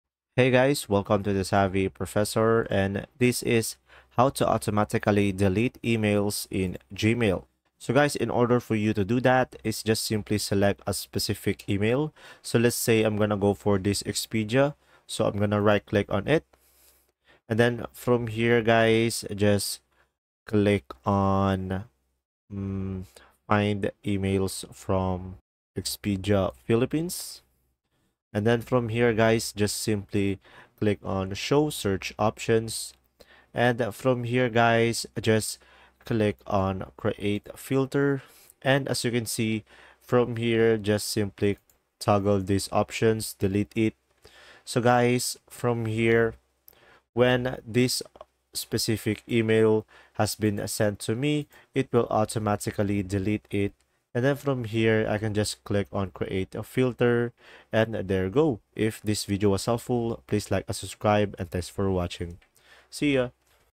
How To Automatically Delete Emails In Gmail (How Can You Set Gmail To Auto Delete Emails?)
How To Automatically Delete Emails In Gmail (How Can You Set Gmail To Auto Delete Emails?). In this video tutorial I will show you how to automatically delete emails in Gmail.